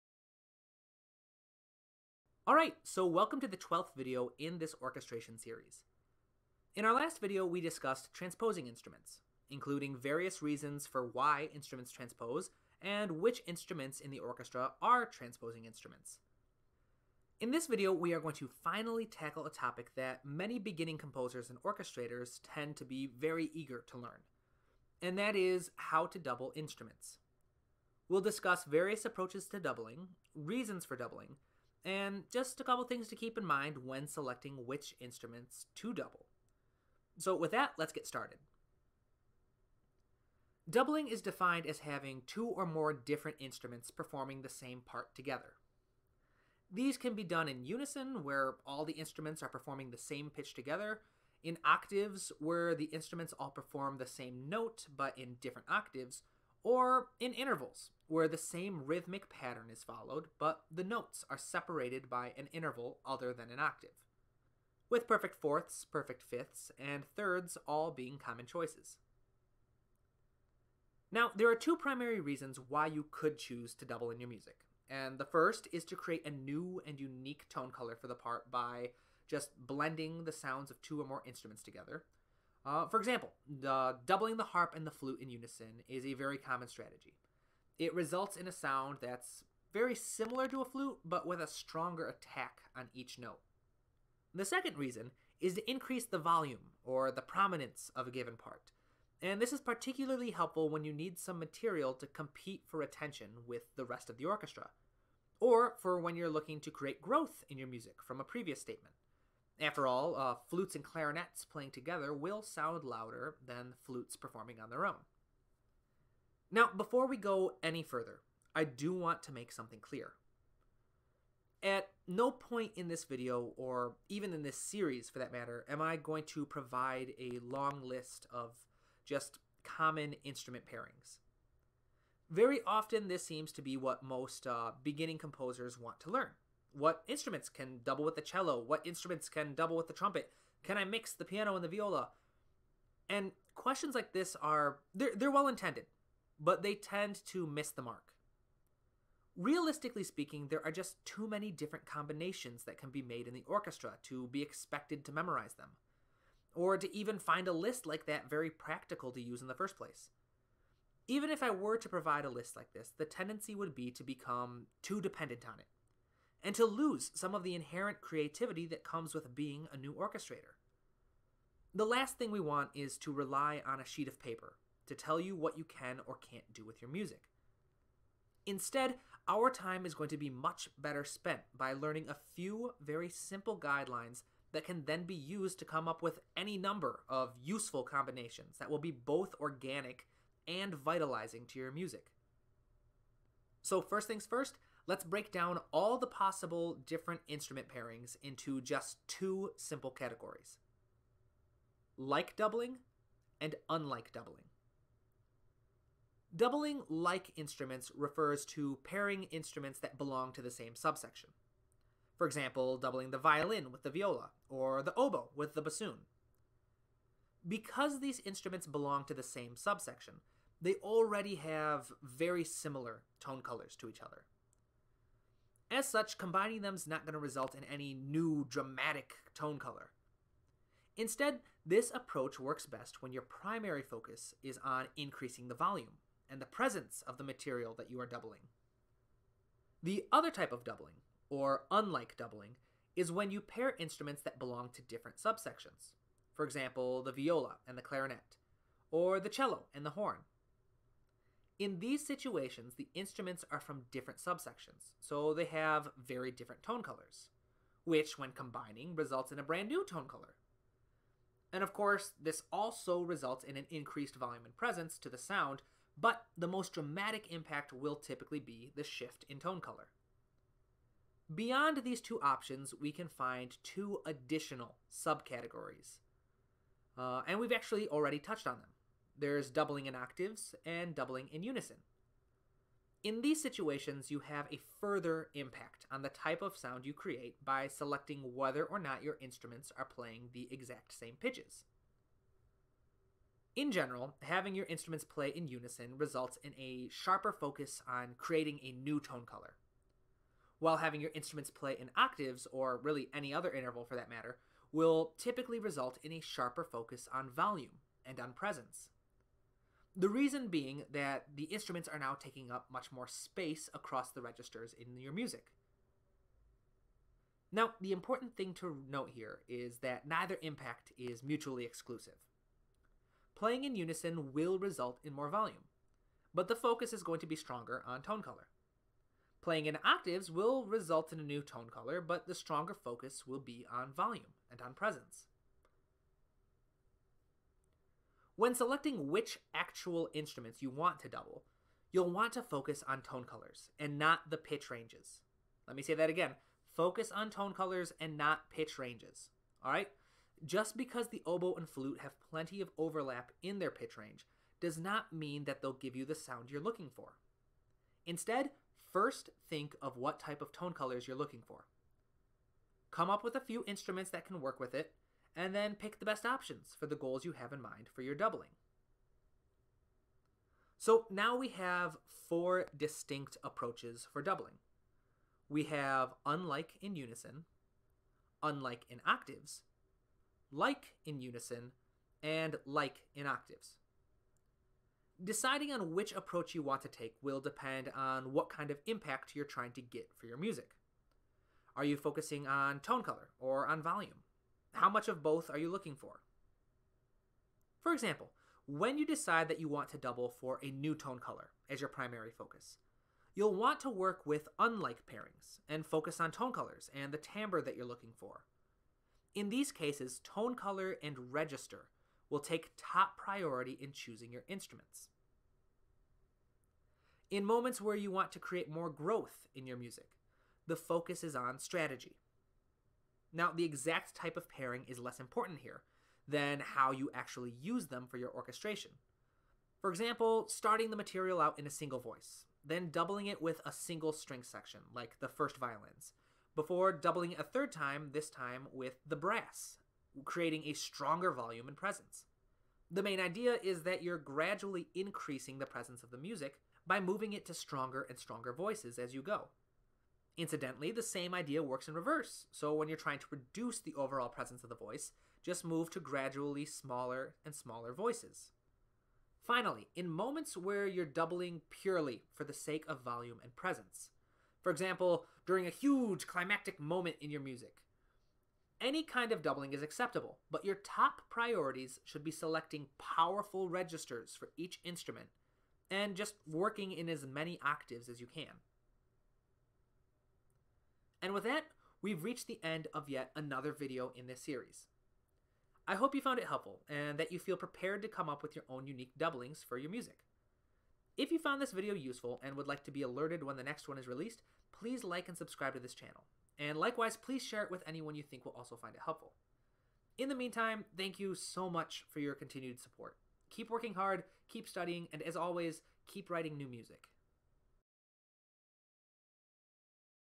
Orchestration 101: Doubling Instruments
In this video, we’ll discuss various approaches to doubling instruments. We'll also cover reasons for doubling, and things to keep in mind when selecting which instruments to pair together.